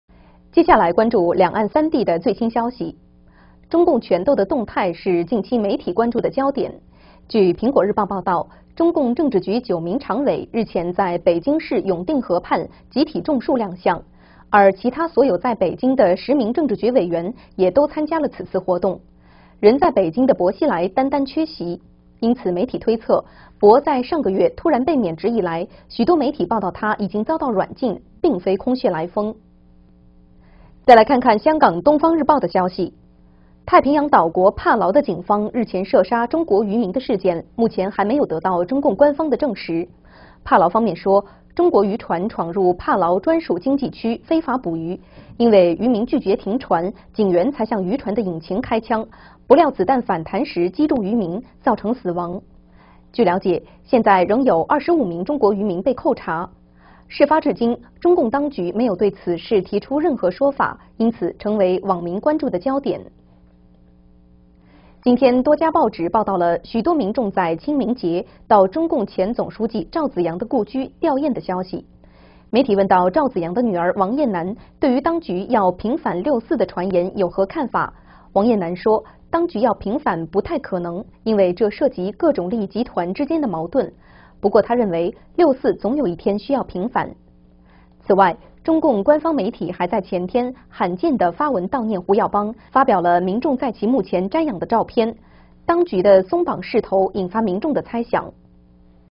九常委權鬥後集體亮相 薄熙來軟禁缺席
【新唐人2012年4月5日訊】最新報導。
1.九常委權鬥後集體亮相 薄熙來軟禁缺席

接下來看幾則港臺媒體的最新消息，中共權鬥的動態是近期媒體關注的熱點，蘋果日報報導中共政治局九位常委一起到北京市永定河畔，集體種樹亮相，而其他所有在北京的10位政治局委員，也都參加了這次活動，可是獨獨缺了人在北京的薄熙來，因此，媒體推測，薄在上月中突然免職以來，許多媒體報導他已經遭到「軟禁」的消息，並非空穴來風。

2.漁民1死25被扣 帕勞衝突當局不作為

來看香港東方日報的消息，太平洋島國帕勞警方日前射殺中國漁民的事件，到現在還沒有得到中共官方證實。帕勞方面說，中國漁船闖入帕勞專屬經濟區算是非法捕魚，因為漁民拒絕停船，警員才向漁船引擎開槍，不料子彈反彈時擊中漁民，才造成死亡。據了解，現在仍有25名中國漁民被扣查。這起事件成了中國網民關注的焦點，因為至今當局都沒有對這件事情提出說法。

3.趙紫陽女：六四總有一天平反

今天許多報紙都報導了許多民眾在清明節，到前中共總書記趙紫陽故居弔唁的消息，媒體問到趙紫陽女兒王雁南，對於當局要平反六四的傳言，王雁南說，當局要平反「不太可能」，因為這涉及各種利益各種集團之間的利益矛盾。不過她認為，六四總有一天需要平反。

我們觀察到，在這次在清明前夕湧入數百名祭拜人潮，其中還包括六四事件領袖。此外，官方媒體還在前天罕見發文悼念胡耀邦，發表了民眾在墓前瞻仰的照片，這次當局鬆綁的作風引發聯想。